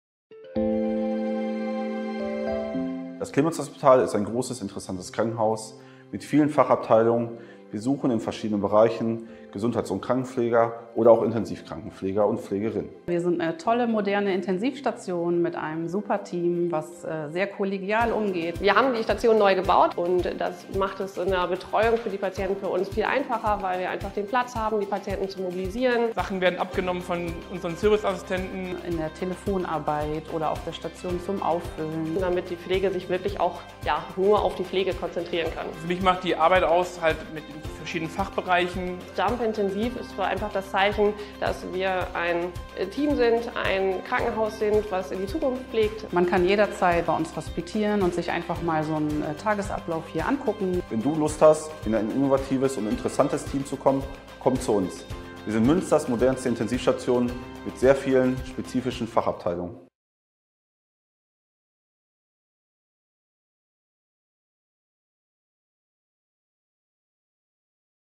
Clemenshospital: Arbeiten auf der Intensivstation für Erwachsene
Intensivpflege bedeutet, sich jeden Tag neuen Herausforderungen zu stellen, hier ist kein Tag wie der andere. Auf der neuen, modernen Intensivstation des Clemenshospitals macht das Arbeiten in einem motivierten Team einfach Spaß!